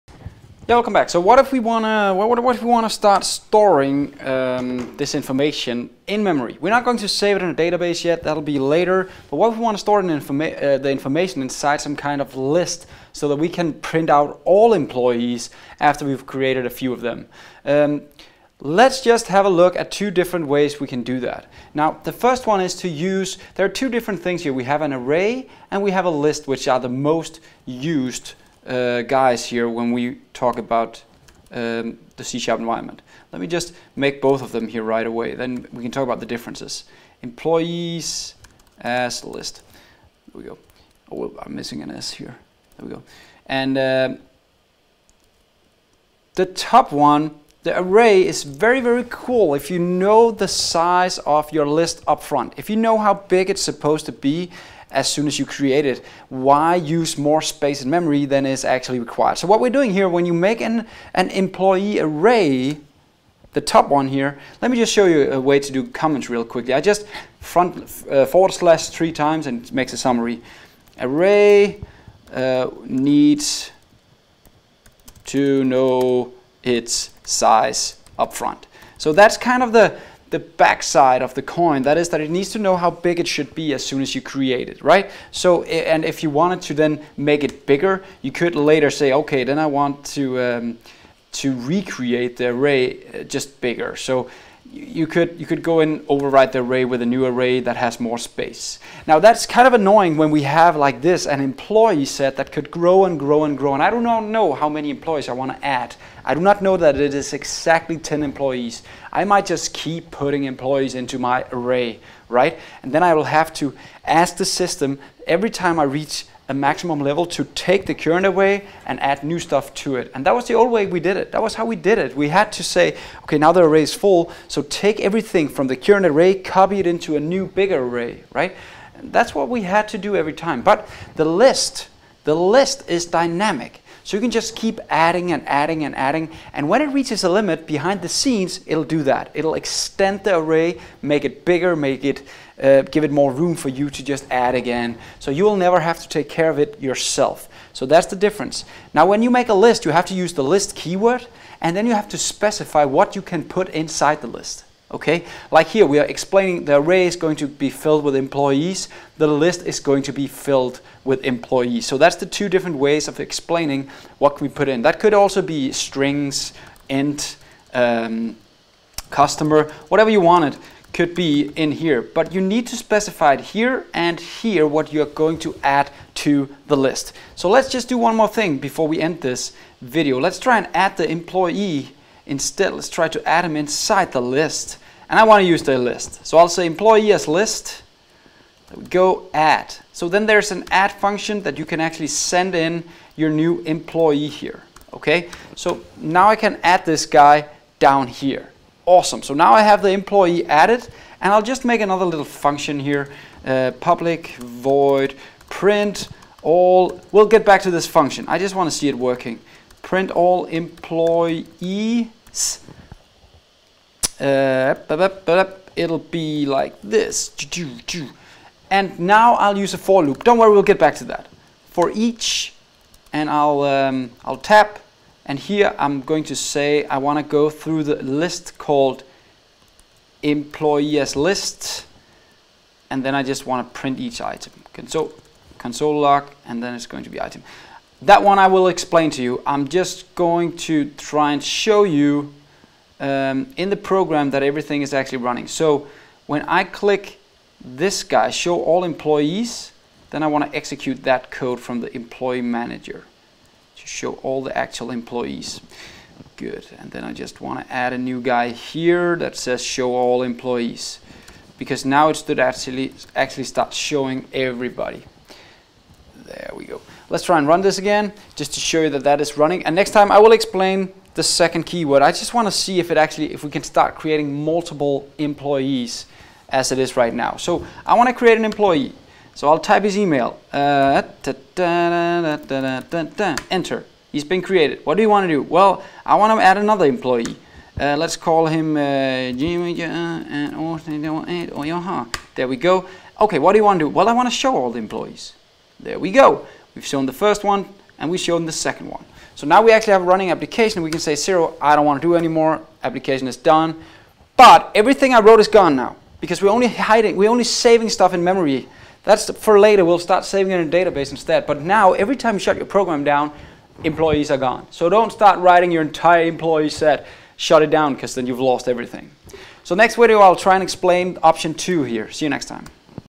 Lists VS Arrays
We will look briefly at the difference between generic lists and arrays.